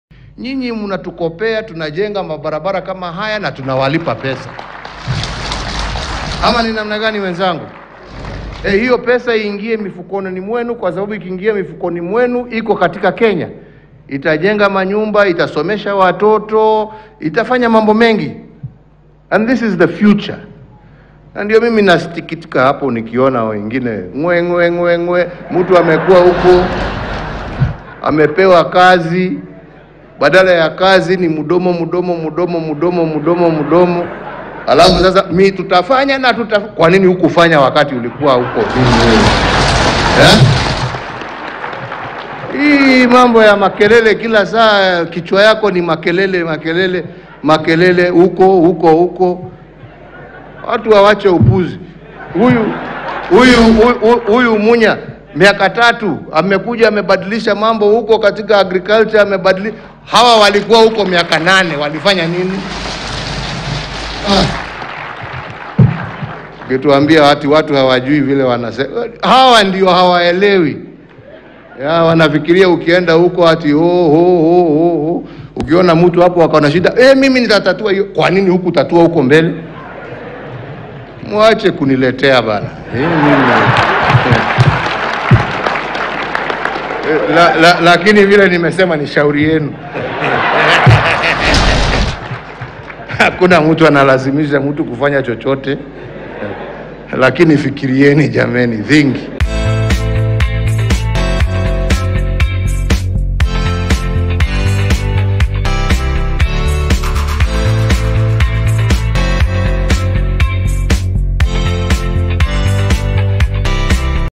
“MUACHE KUNILETEA BWANA! KICHWA YAKO NI MAKELELE KILA SAA.” UHURU CHIDES RUTO
President Uhuru Kenyatta presided over Ushirika day celebrations held at KICC and gave veiled but direct attacks on the Kenya Kwanza brigade led by DP Ruto.